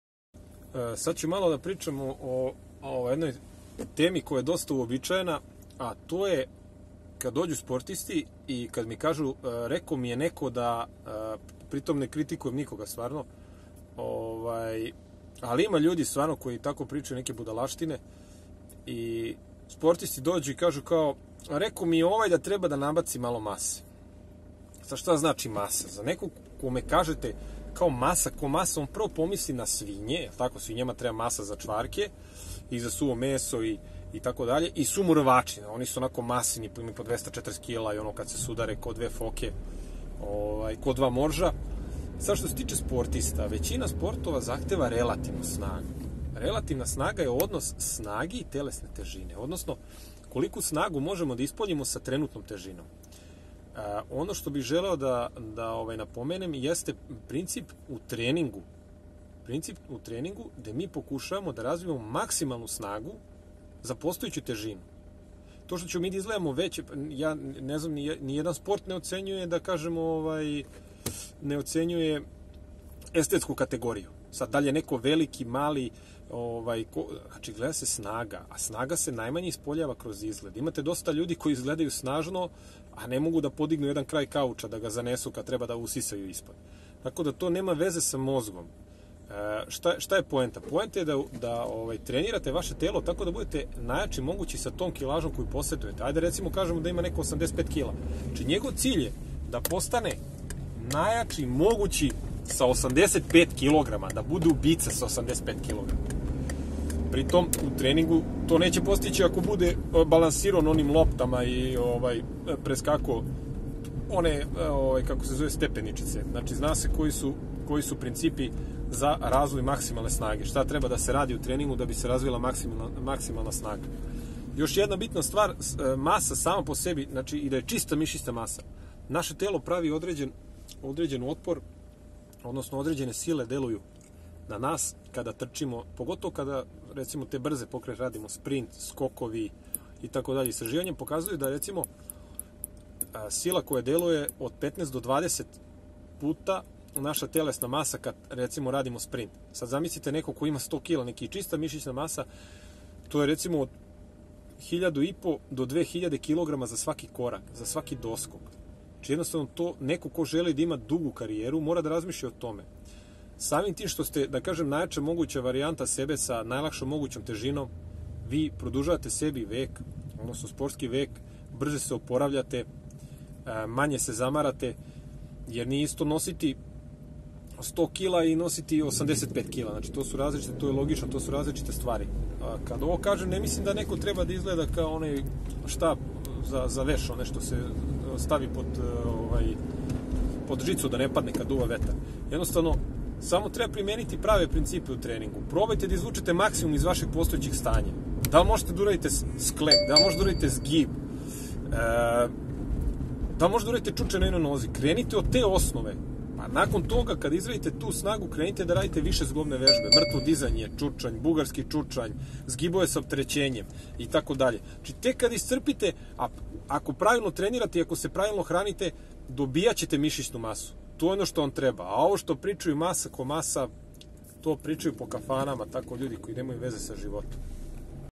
Kad ti neko kaže da treba da nabaciš masu🤔
Dosta česta pojava da dobiješ savet od "stručnjaka"da treba da nabaciš masu da bi kao bio bolji u sportu koji igraš.Pri tome niko ne spominje snagu,kao ni odnos snage i telesne težine.Nabacivanje mase bez  napretka u snazi apsolutno nema nikakve svrhe.
Zdrav razum kaže da bi za svaki kilogram mišićne mase trebao da postigneš veći nivo snage.Probaj da postaneš najača moguća verzija sebe sa postojećom kilažom.Kako se to postiže?Pa primenom metoda u treningu koji razvijaju maksimalnu snagu.Ali pre toga treba da diplomiraš bodyweight vežbe i tehniku njihovog izvođenja.Ne preskači stepenice u razvoju snage, da bi zaslužio da legneš na bendž klupu prvo moraš da budeš sposoban da pravilno izvodiš sklekove.To isto važi i za ostale višezglovne vežbe.Obično savet "treba ti masa"služi kao pokriće za neznanje.Ipak u pitanju je tvoja karijera i tvoj život tako da svakako zaslužuješ mnogo bolji savet od treba ti masa!